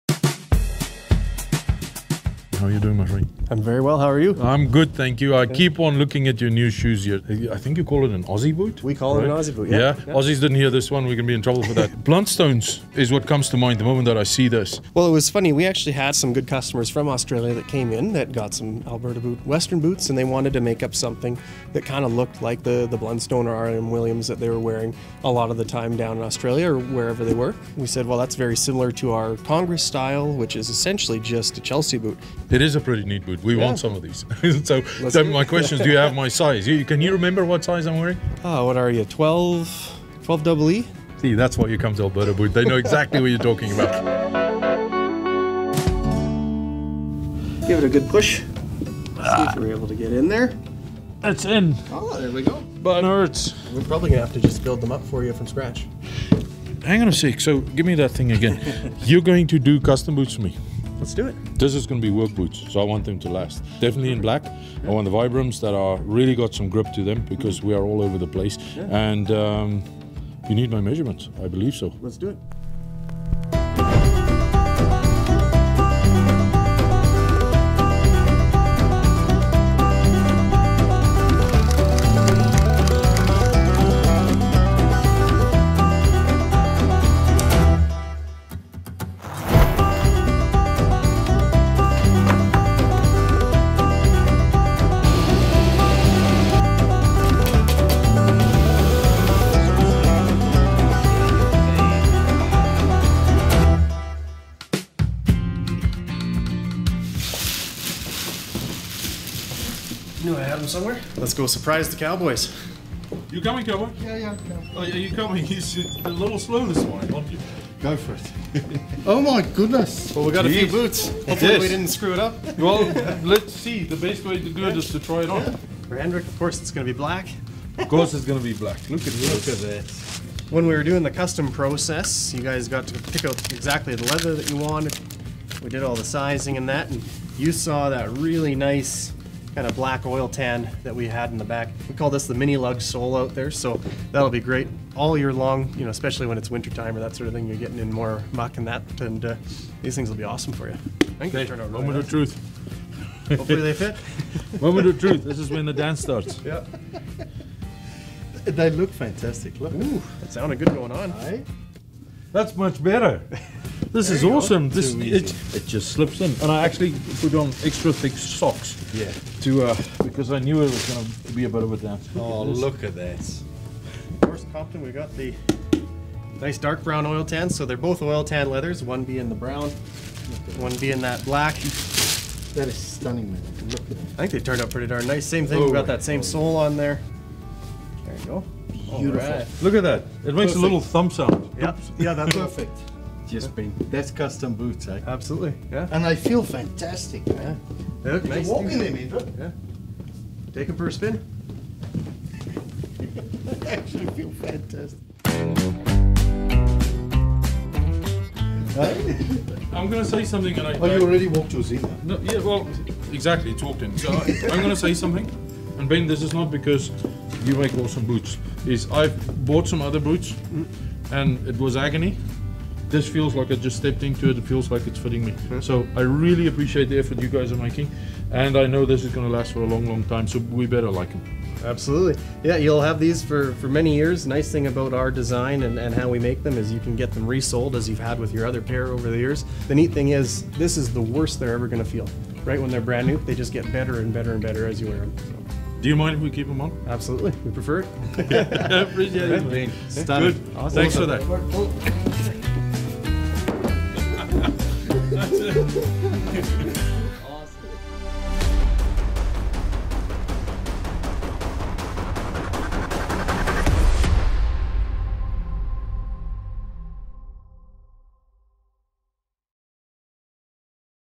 Two Cowboys: Custom “Aussie” Boots for the Cowboys from Alberta Boot Company, Calgary, Alberta
We are featuring: Alberta Boot Company Custom Boots for the Cowboys

Ben Gerwing is a master bo ot maker that learned from his grandfather, the founder of Alberta Boot Company. Today he is continuing the legacy with some new designs. However, old fashioned customer service and custom boot making is still integral to what makes the business unique.

Ben made some custom “Aussie” style boots for the Two Cowboys because our feet does not fit the norm. Thank you to Ben and his team for keeping us in our Alberta Boots, even in warmer weather.

This is who we are:

We are the Two Cowboys that are travelling the world, in search of people that make things. We showcase the awesomeness in communities all over Canada, U.S., and New Zealand.